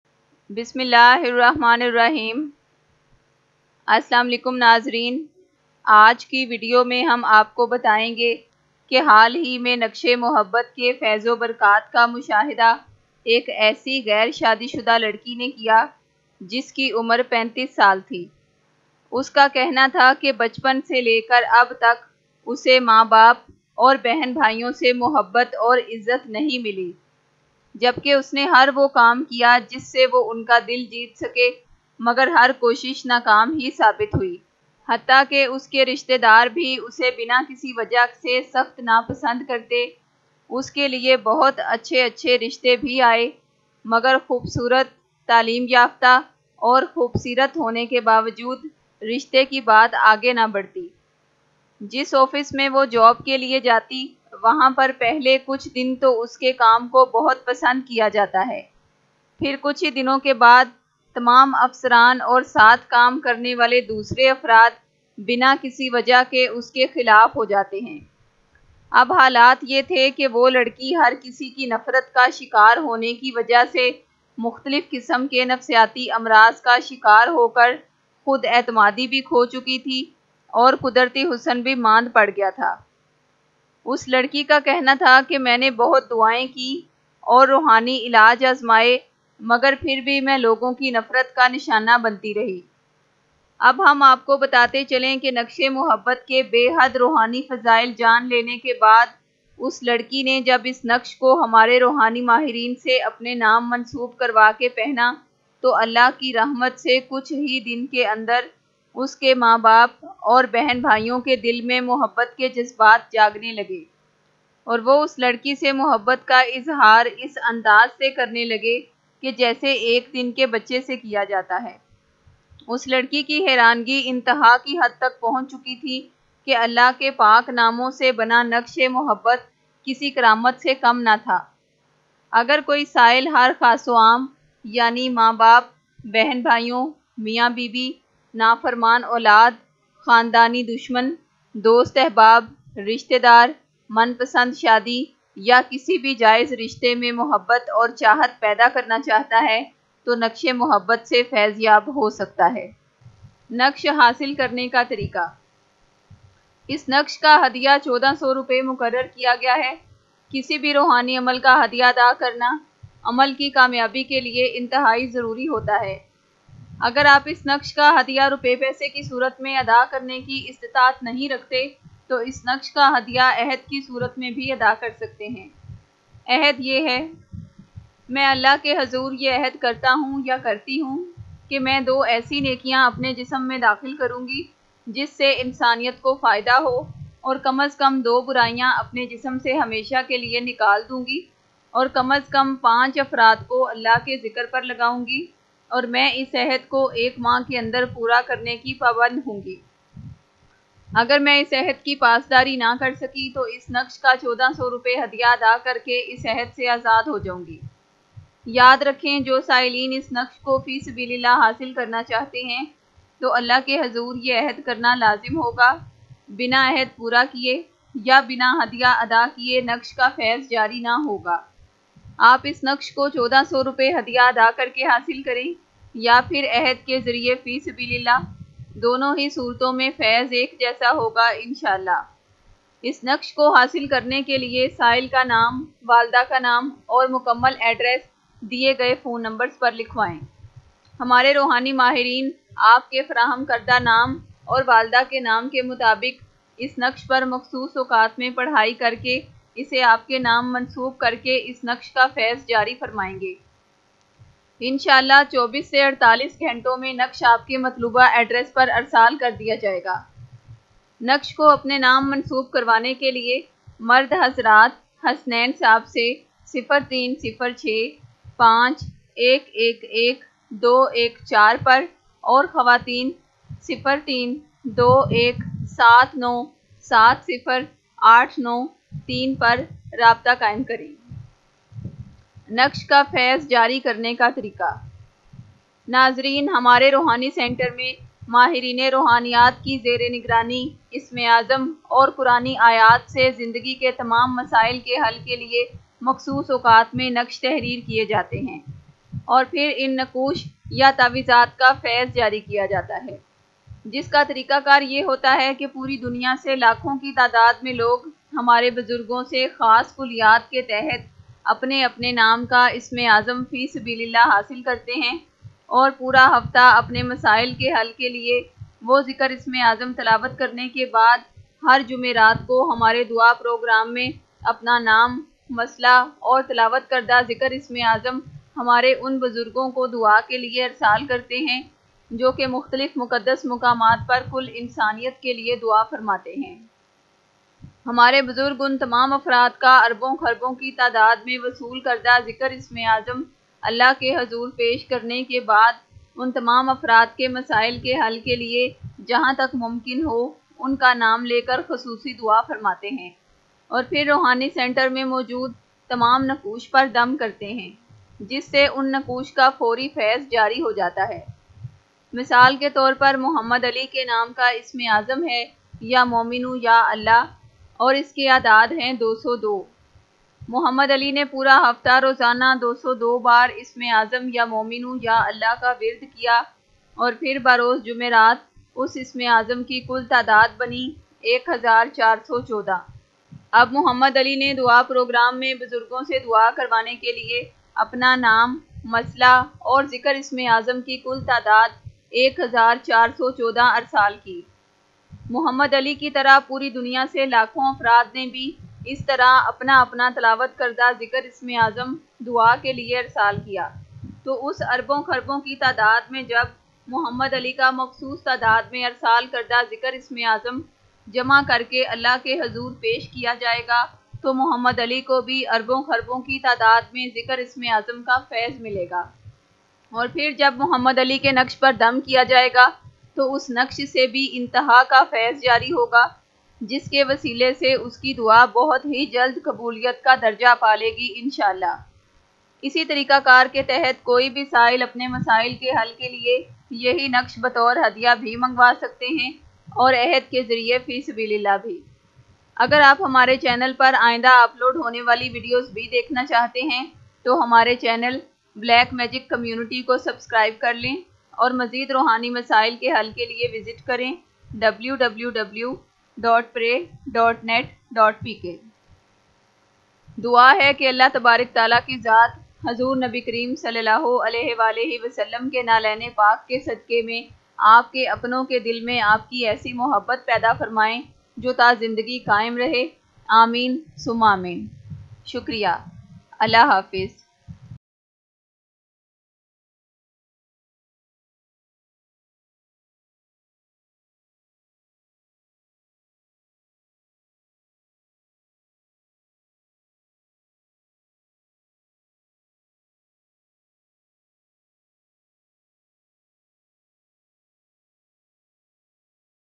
Taweez e Mohabbat
Aaj ki video Taweez e Mohabbat main ham apko aisi larki ka waqia bata rahe hain jiski zindagi Taweez e Mohabbat ki barkat se sirf 7 din k andr badal gai. Usay nafrat karnay walay tamam afrad k dil main uski bay inteha mohabbat paida hoi. Mazeed details k liye ap is Video Taweez e Mohabbat ko dekhen.